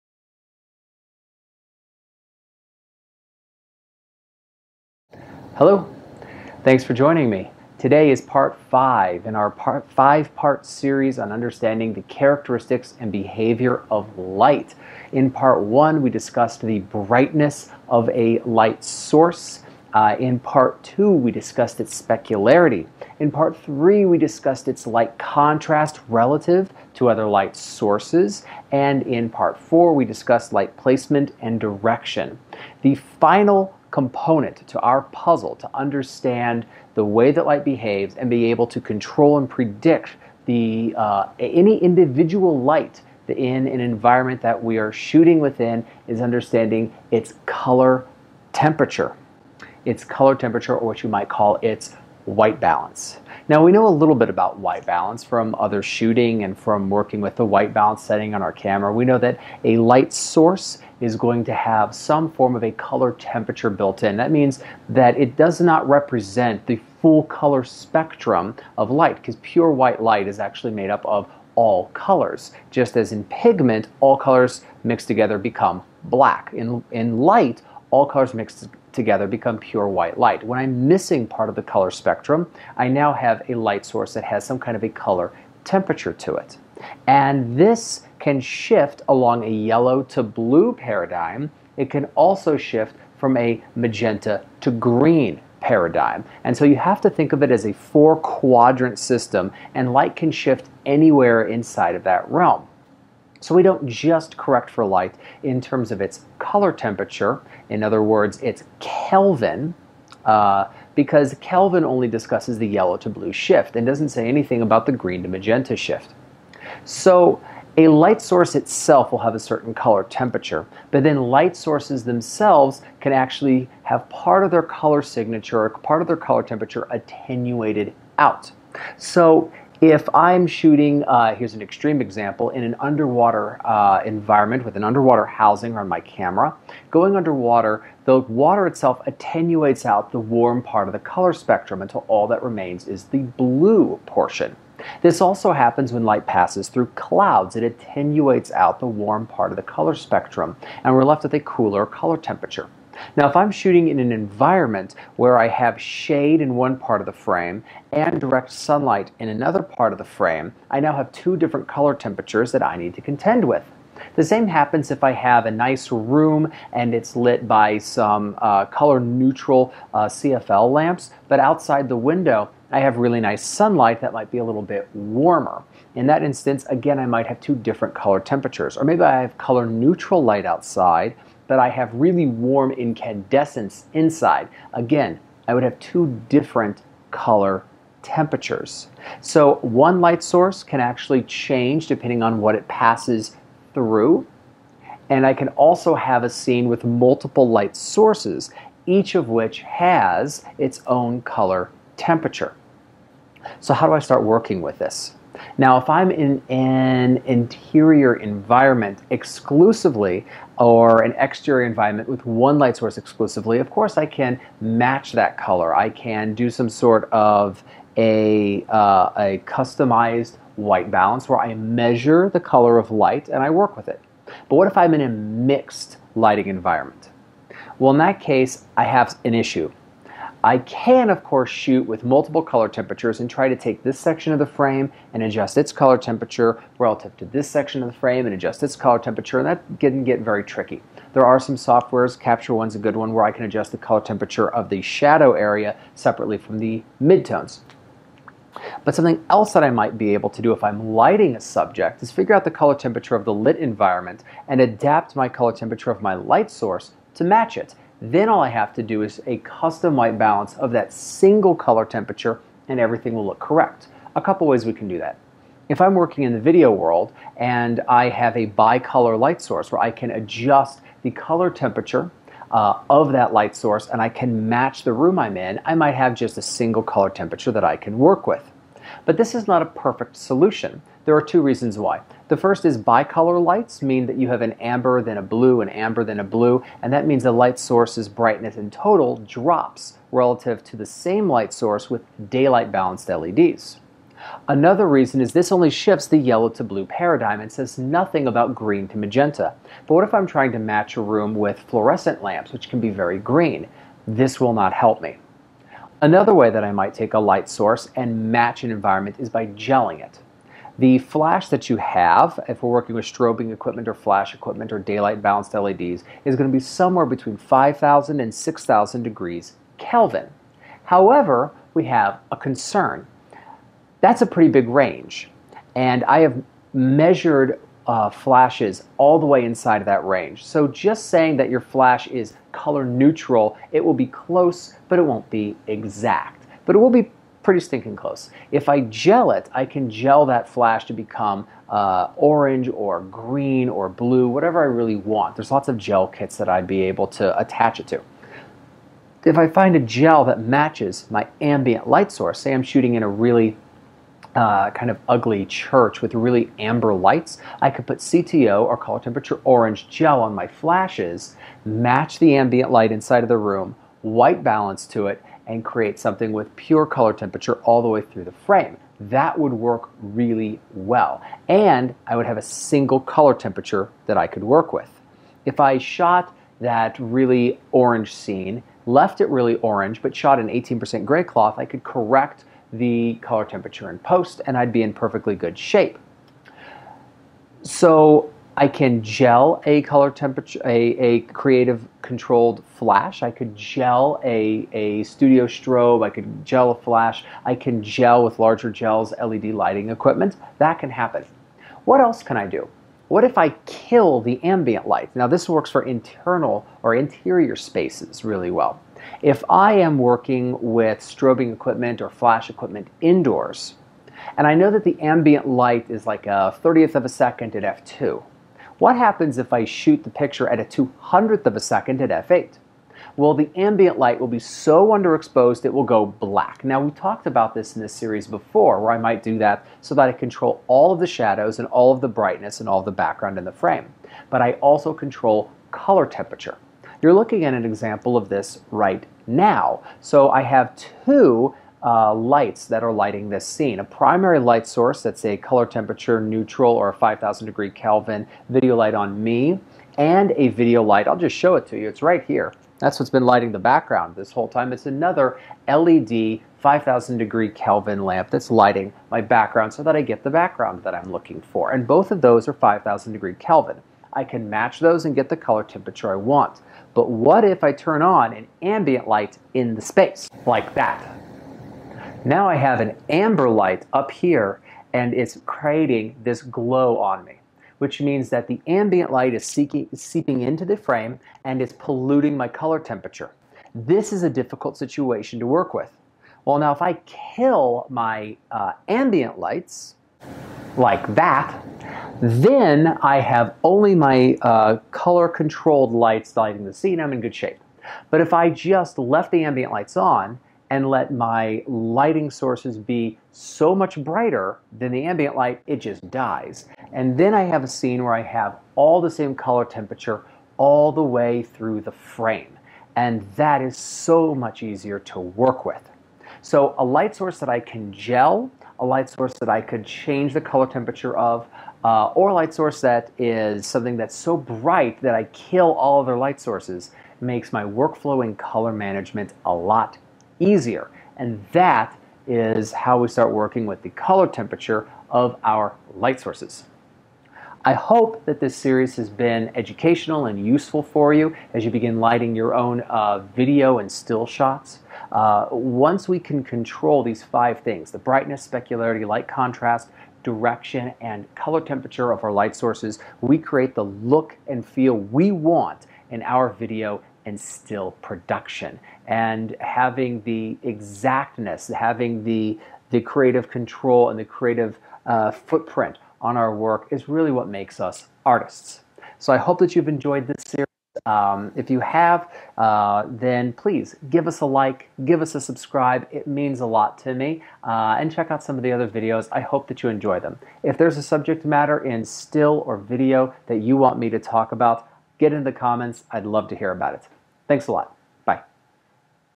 Characteristics of Light, Part 5: Color Temperature
Most photographers do not have a mental framework for understanding strobes, flashes, and lit exposures. Over the course of five videos we are going to break down the behavior of light into five characteristics and understand each in turn. Today, we're talking about color temperature.